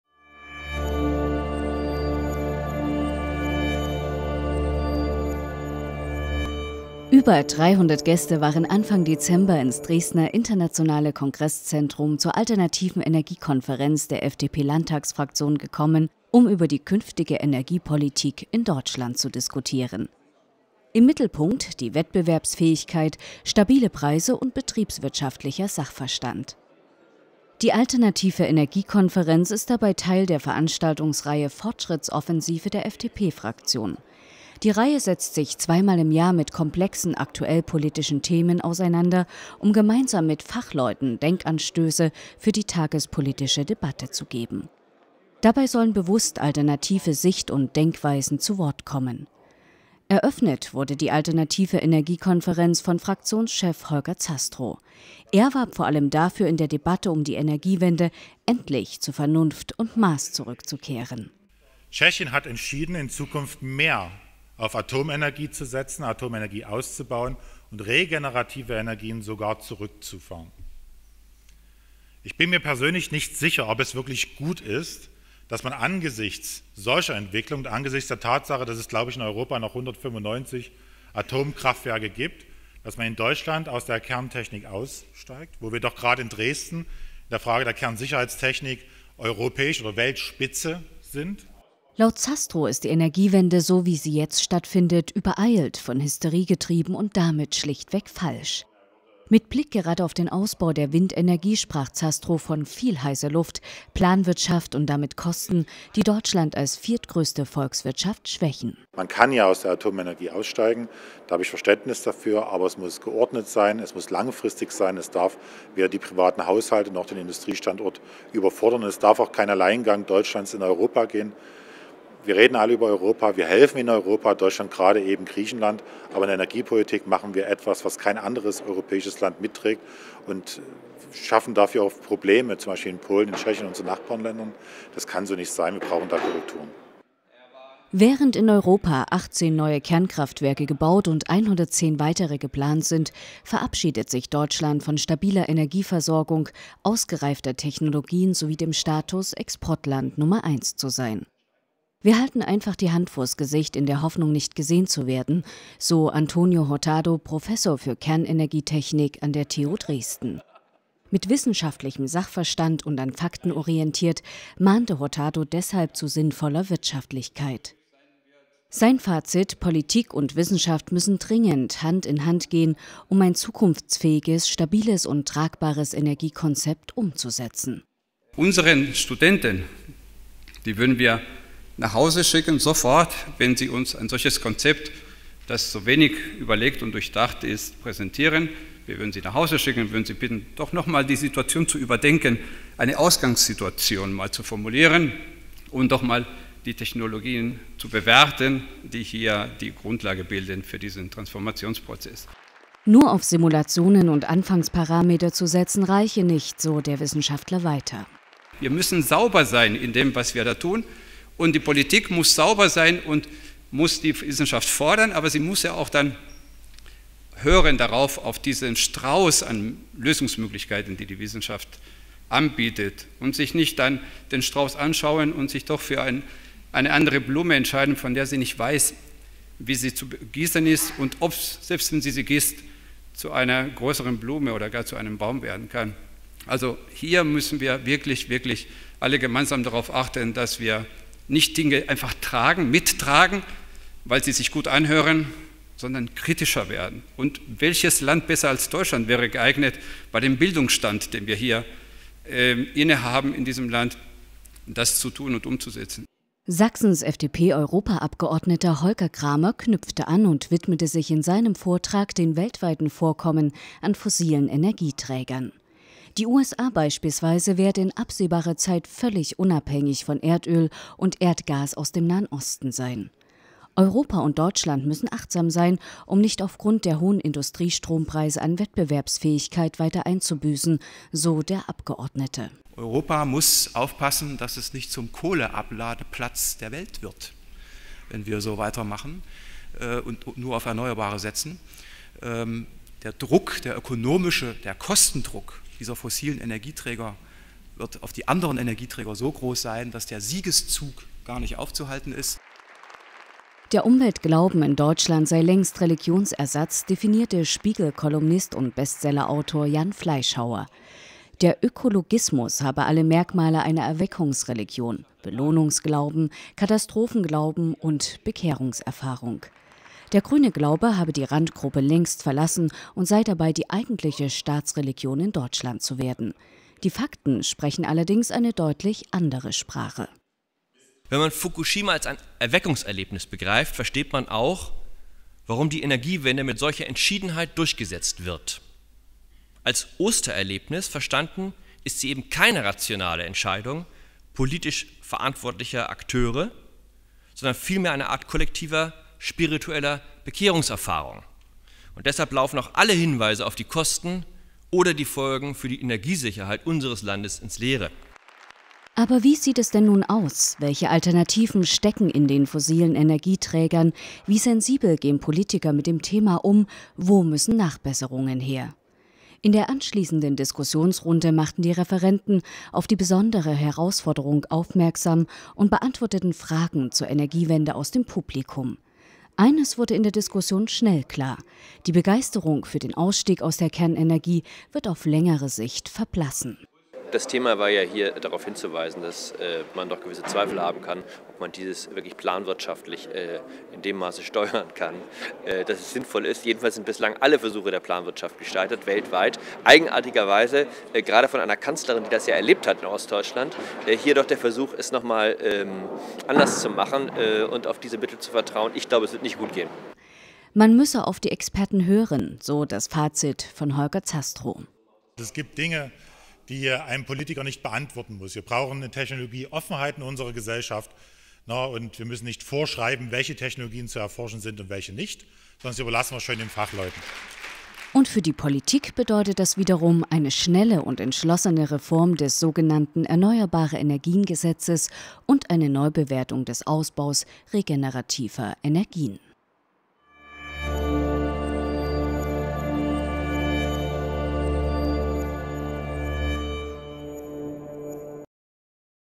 Fortschrittsoffensive - Alternative Energiekonferenz der FDP-Fraktion im Sächsischen Landtag
Über 300 Teilnehmer bei Alternativer Energiekonferenz in Dresden

„Ich würde meine Studenten nach Hause schicken und ihnen empfehlen, noch mal über die Wahl ihres Studienganges nachzudenken, wenn sie mir ein solch undurchdachtes Experiment wie die Energiewende als Konzept anbieten würden", das sagt Prof. Dr. Antonio Hurtado, Direktor des Institutes für Energietechnik der TU Dresden. Der gebürtige Spanier war einer der Referenten der Alternativen Energiekonferenz am 01. Dezember in Dresden. Die Veranstaltung mit über 300 Besuchern ist Teil der „Fortschrittsoffensive" der FDP-Landtagsfraktion. Nach der Alternativen Klimakonferenz im Sommer stand dabei diesmal die Energiepolitik in Deutschland im Mittelpunkt.

Eröffnet wurde die Konferenz unter dem Titel „Zurück in die Steinzeit -- Wie viel Energiewende verträgt Deutschland?" von FDP-Landes- und Fraktionschef Holger Zastrow. Die politische Entscheidung zur „Energiewende" sei nicht aus Vernunft und auf der Basis wissenschaftlicher Erkenntnisse, sondern aus parteipolitischem Kalkül getroffen worden. Er forderte dazu auf, in der Energiedebatte zu Vernunft und Maß zurückzukehren. Was man derzeit stattdessen erlebe, sei nichts anderes als ein energiepolitischer Blindflug.

Nach der politischen Einschätzung durch Holger Zastrow war es TU-Professor Antonio Hurtado dann vorbehalten, die „Energiewende" nach streng wissenschaftlichen Maßstäben zu bewerten. Die sei physikalisch unmöglich, auch wenn die Politik es wolle. „Wir halten die Hand vors Gesicht in der Hoffnung, nicht gesehen zu werden", fasste Hurtado sein Urteil zusammen. Sachsens FDP-Europaabgeordneter Holger Krahmer machte klar, dass der aktuelle Trend zu den fossilen Brennstoffen gehe. So würden etwa die USA in absehbarer Zeit unabhängig von Erdöl und Erdgas aus dem Nahen Osten sein. „Deutschland dagegen hat inzwischen die höchsten Industriestrompreise weltweit -- eine Gefahr für die Wettbewerbsfähigkeit."

Daniel Wetzel, Journalist und Energieexperte der WELT-Zeitungsgruppe, machte deutlich, dass die Zukunftsfähigkeit des EEG auch in Hintergrundkreisen in Berlin sowohl von Experten als auch Verbänden kritisch gesehen wird. Denn sonst setze man die industrielle Basis unserer Volkswirtschaft wie bei einer gigantischen Wette aufs Spiel. Dr. Philip Plickert, Wirtschaftsredakteur der Frankfurter Allgemeinen Zeitung, nahm sich den „wissenschaftlich angekündigten Weltuntergang" des „Club of Rome" und dessen neueste Vorhersagen vor. Genauso wenig wie das für 1990 angekündigte Ende des Erdölzeitalters eingetroffen ist, werde der nun für 2050 vorhergesagte „Weltuntergang" passieren.

Spiegelredakteur und Bestseller-Autor Jan Fleischhauer wies launig nach, dass der Umweltglauben eine Ersatzreligion ist. Der „Ökologismus" habe alle Merkmale -- Belohnungs- und Katastrophenglauben und das gemeinsame Bekehrungserleben „Energiewende". Deshalb seien die Ökologismusgläubigen auch unzugänglich für rationale Argumente. Sachsens Wirtschaftsminister Sven Morlok machte schließlich im Gespräch mit dem stellvertretenden Chefredakteur der Wirtschaftswoche, Henning Krumrey, deutlich, dass das Erneuerbare Energiegesetz (EEG) noch vor der Bundestagswahl reformiert werden muss. Die Widerstände seien aber groß, denn „den Protagonisten der Erneuerbaren Energien geht es nicht um die Erneuerbaren Energien, sondern um die Beibehaltung eines profitablen Wirtschaftsmodells."

Teilnehmer der Konferenz:

Holger Krahmer MdEP, Umweltpolitischer Sprecher der FDP im Europäischen Parlament
Prof. Dr.-Ing. habil. Antonio Hurtado, Direktor des Institutes für Energietechnik der TU Dresden
Daniel Wetzel, Wirtschaftsredakteur und Energieexperte der Welt-Gruppe, Berlin
Dr. Philip Plickert, Wirtschaftsredakteur der Frankfurter Allgemeinen Zeitung
Jan Fleischhauer, SPIEGEL-Kolumnist und Bestsellerautor
Holger Zastrow MdL, Vorsitzender der FDP-Fraktion im Sächsischen Landtag und der FDP Sachsen
Sven Morlok MdL, Sächsischer Staatsminister für Wirtschaft, Arbeit und Verkehr, im Gespräch mit
Henning Krumrey, stellv. Chefredakteur WirtschaftsWoche